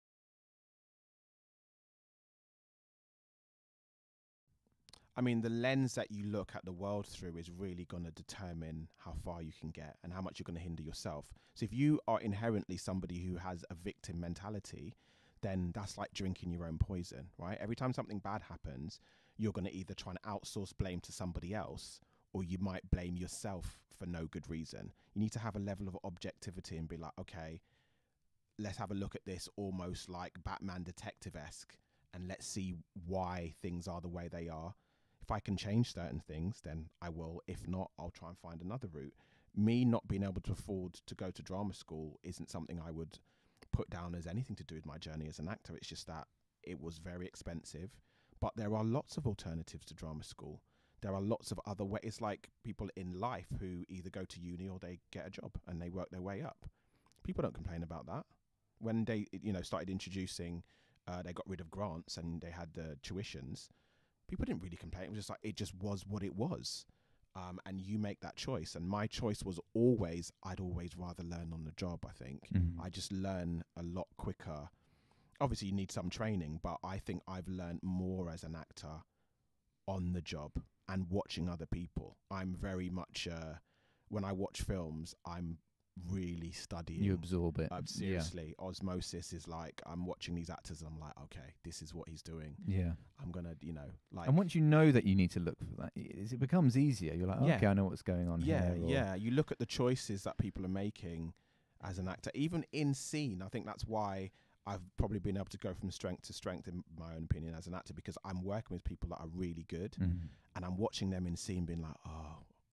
Hainsley Lloyd Bennett | Importance of Mindset to Breaking Down Barriers | Crossing the Line Podcast
Hainsley Lloyd Bennett is a British actor known for his work in shows such as 'Washington', 'Catastrophe' and most recently as Deon 'Bazza' Bashford in Starzplay's 'Pennyworth'.